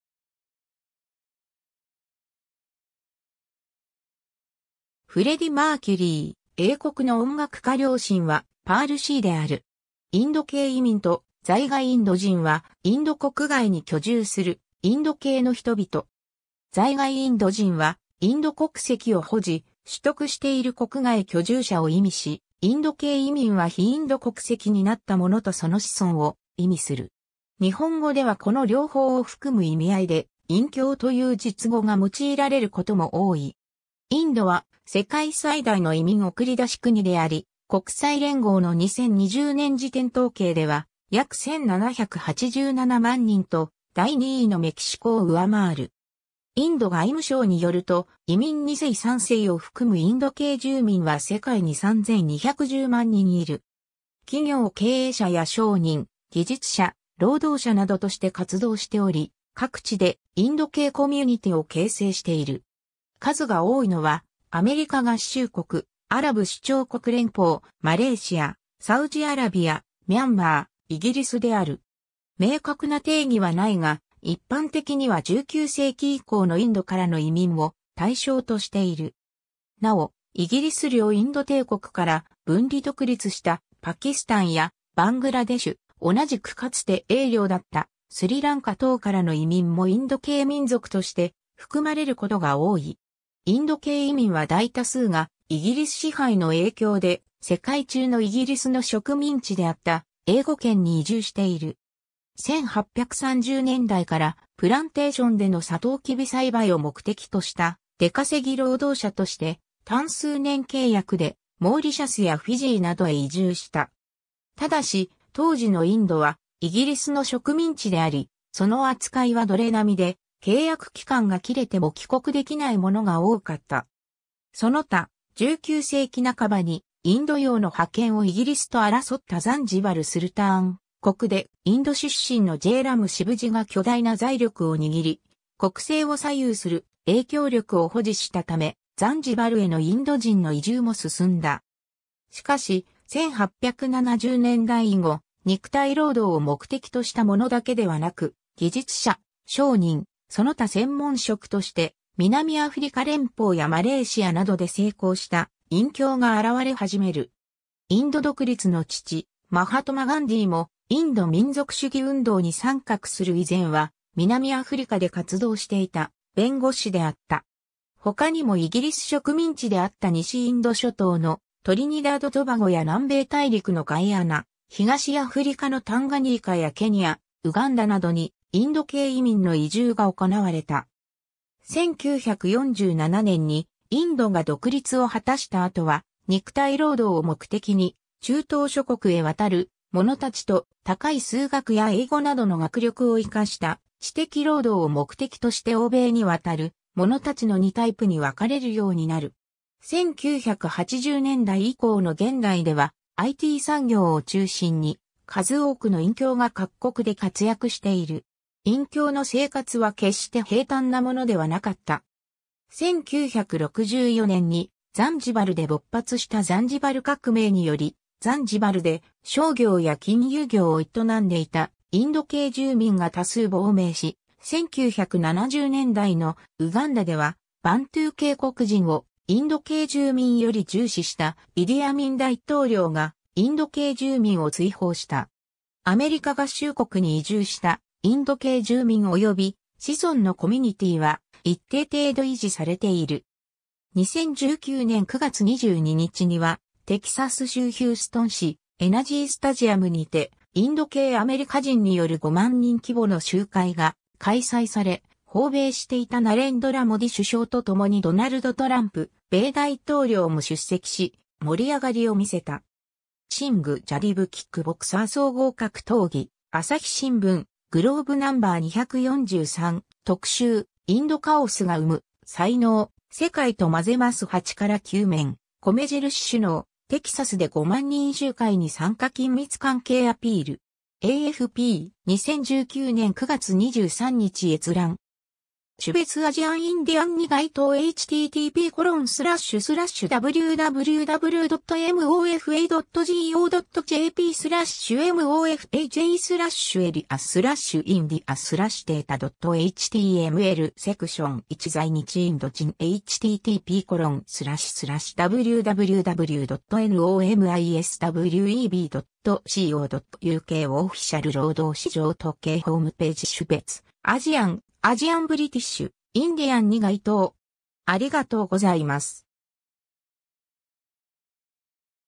インド系移民と在外インド人
フレディ・マーキュリー、英国の音楽家 両親はパールシー（ペルシャ系インド人）である。
 インド系移民（インドけいいみん、People/Persons of Indian Origin, PIO）と在外インド人（ざいがいインドじん、非居住インド人、Non-Resident Indians, NRI, 総称してNRI/PIOとも）は、インド国外に居住するインド系の人々。
在外インド人（NRI）はインド国籍を保持・取得している国外居住者を意味し、インド系移民（PIO）は非インド国籍になった者とその子孫を意味する。
日本語ではこの両方を含む意味合いで「印僑」（いんきょう）という術語が用いられることも多い。
 インドは世界最大の移民送り出し国であり、国際連合の2020年時点統計では約1787万人と第2位のメキシコ（約1119万人）を上回る。
インド外務省によると、移民2世・3世を含むインド系住民（ディアスポラ）は世界に3210万人いる。
企業経営者や商人、技術者、労働者などとして活動しており、各地でインド系コミュニティを形成している。
数が多いのはアメリカ合衆国（446万人）、アラブ首長国連邦（342万人）、マレーシア（298万人）、サウジアラビア（259万人）、ミャンマー（200万人）、イギリス（176万人）である。
 明確な定義はないが、一般的には19世紀以降のインドからの移民を対象としている。
なお、イギリス領インド帝国から分離独立したパキスタンやバングラデシュ、同じくかつて英領だったスリランカ等からの移民もインド系民族として含まれることが多い。
インド系移民は大多数がイギリス支配の影響で、世界中のイギリスの植民地であった英語圏に移住している。
 1830年代から、プランテーションでのサトウキビ栽培を目的とした出稼ぎ労働者として、単数年契約でモーリシャスやフィジーなどへ移住した。
ただし、当時のインドはイギリスの植民地であり、その扱いは奴隷並みで契約期間が切れても帰国できない者が多かった。
その他、19世紀半ばにインド洋の覇権をイギリスと争ったザンジバル・スルターン国でインド出身のジェイラム・シヴジが巨大な財力を握り、国政を左右する影響力を保持したため、ザンジバルへのインド人の移住も進んだ。
しかし、1870年代以後、肉体労働を目的としたものだけではなく、技術者、商人、その他専門職として南アフリカ連邦（現在の南アフリカ共和国）やマレーシア（当時はイギリス領マラヤ）などで成功した印僑が現れ始める。
インド独立の父マハトマ・ガンディーも、インド民族主義運動に参画する以前は、南アフリカで活動していた弁護士であった。
他にもイギリス植民地であった西インド諸島のトリニダード・トバゴや南米大陸のガイアナ、東アフリカのタンガニーカやケニア、ウガンダなどにインド系移民の移住が行われた。
 1947年にインドが独立を果たした後は、肉体労働を目的に中東諸国へ渡る者達と、高い数学や英語などの学力を生かした知的労働を目的として欧米に渡る者達の2タイプに分かれるようになる。
1980年代以降の現代ではIT産業を中心に、数多くの印僑が各国で活躍している。
 印僑の生活は決して平坦なものではなかった。
1964年にザンジバルで勃発したザンジバル革命により、ザンジバルで商業や金融業を営んでいたインド系住民が多数亡命し、1970年代のウガンダでは、バントゥー系黒人をインド系住民より重視したイディ・アミン大統領がインド系住民を追放した。
 アメリカ合衆国に移住したインド系住民および子孫のコミュニティーは一定程度維持されている。
2019年9月22日には、テキサス州ヒューストン市NRGスタジアムにてインド系アメリカ人による5万人規模の集会が開催され、訪米していたナレンドラ・モディ首相とともにドナルド・トランプ米大統領も出席し、盛り上がりを見せた。
 シング　ジャディブ　　キックボクサー　総合格闘技 ^ 『朝日新聞』GLOBE（朝刊別刷り）No.243（2021年7月4日）【特集】インド カオスが生む才能／「世界と混ぜます」8-9面 ^ “米印首脳、テキサスで5万人集会に参加 緊密関係アピール”. AFP (2019年9月23日). 2019年9月23日閲覧。
^ 種別"Asian Indian"に該当 ^ "在日インド人" ^ http...